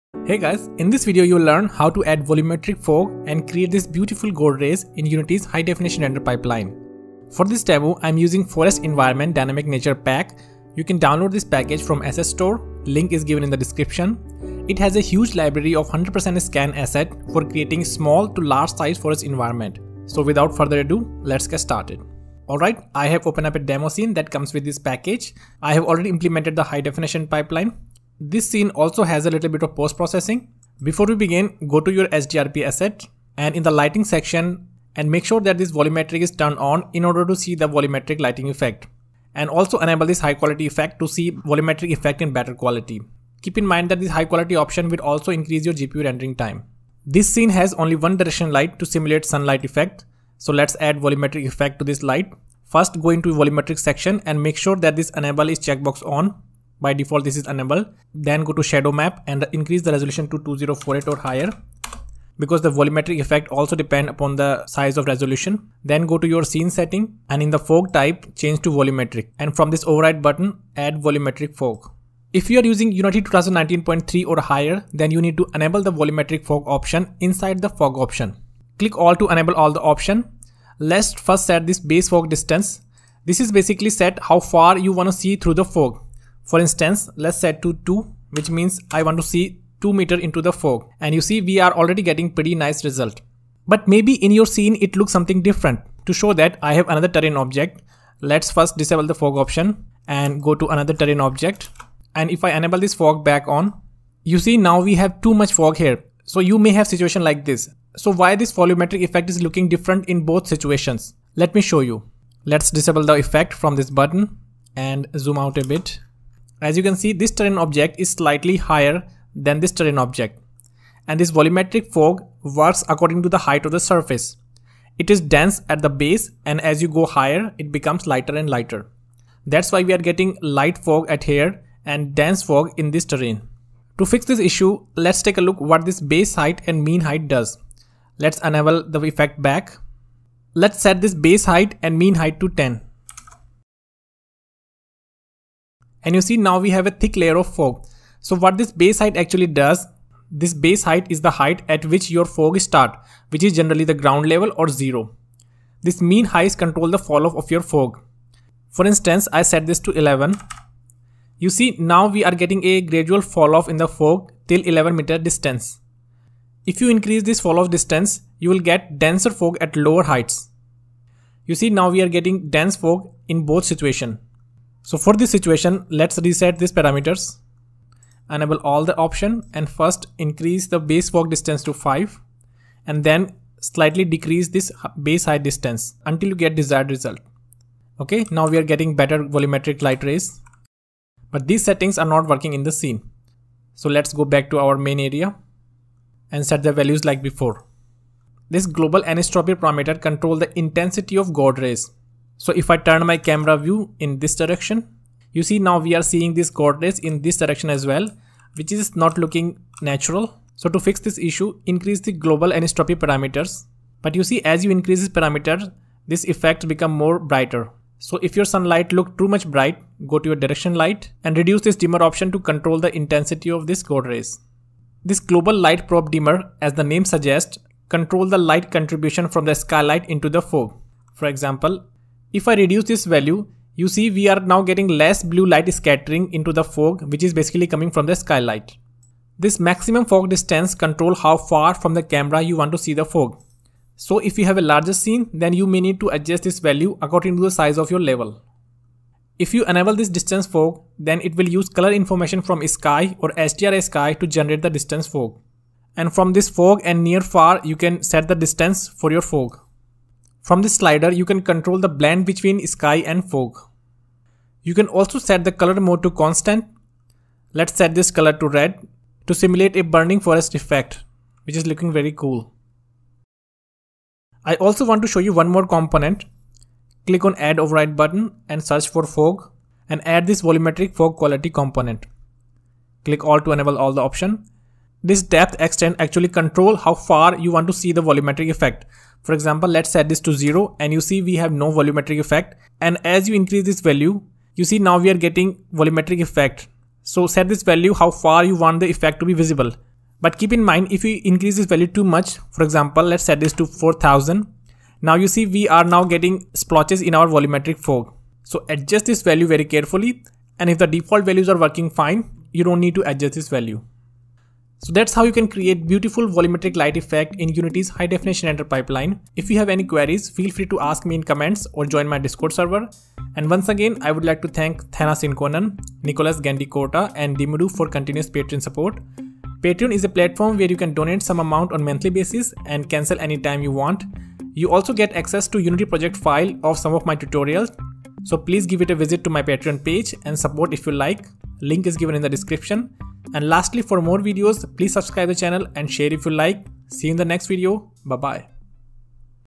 VOLUMETRIC LIGHTING in Unity HDRP
Let's learn how to make god rays using volumetric fog in Unity's High Definition Render Pipeline(HDRP)

HDRP enables you to achieve high quality graphics and image effect in Unity.